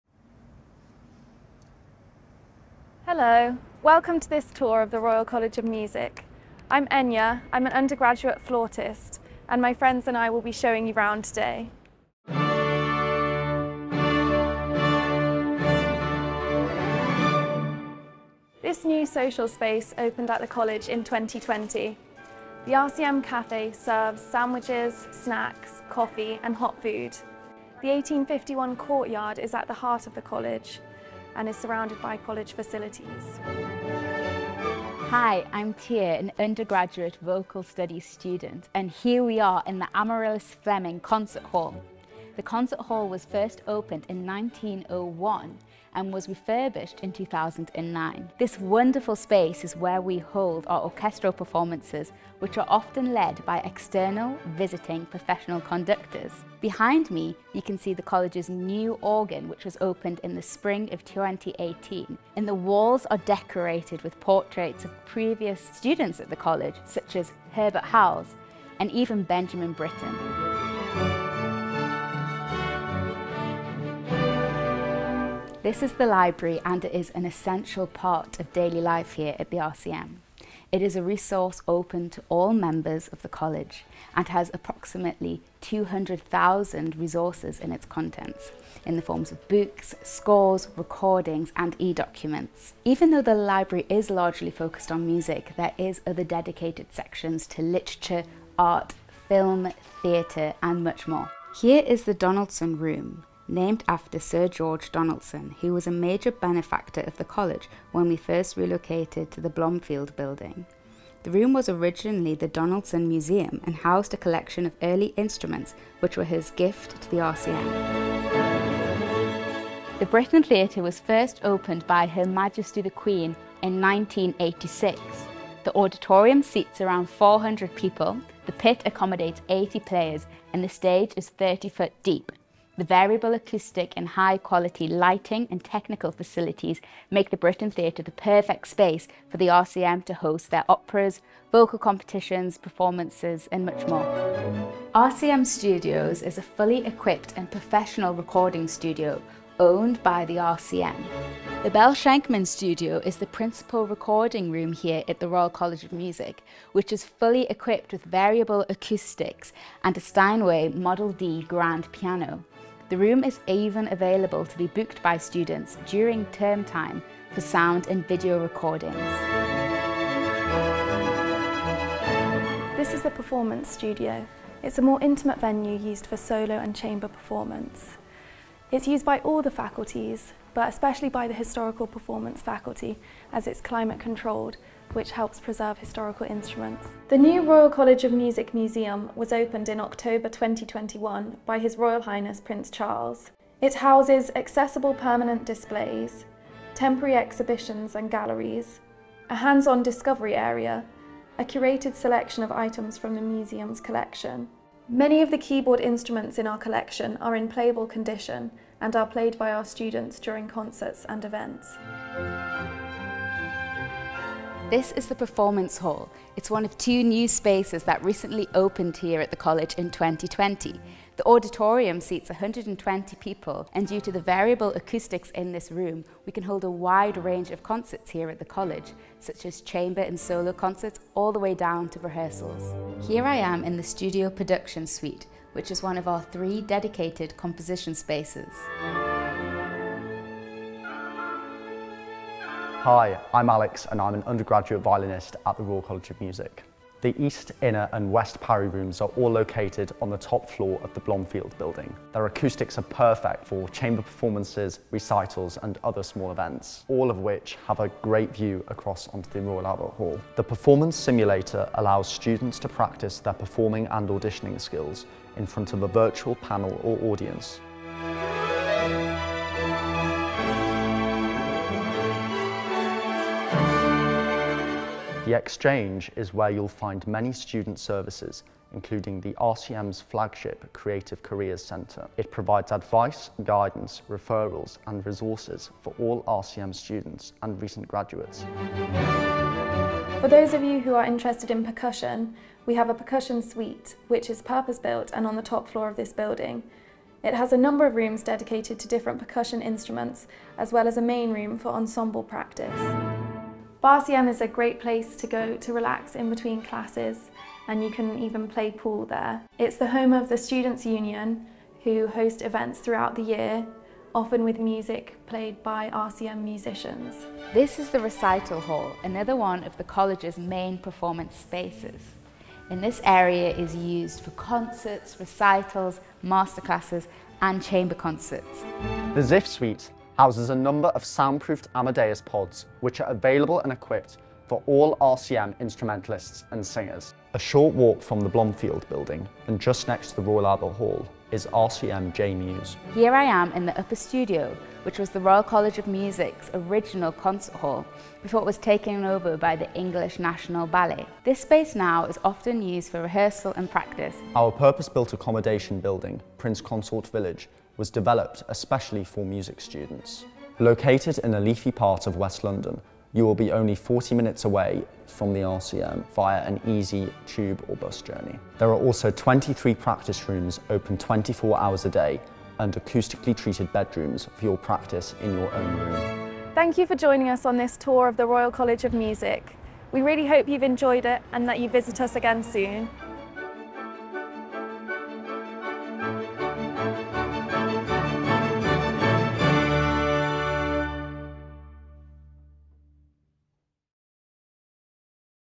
Campus Tour | Royal College of Music
Take a tour of the Royal College of Music's campus and facilities with some of our undergraduate students.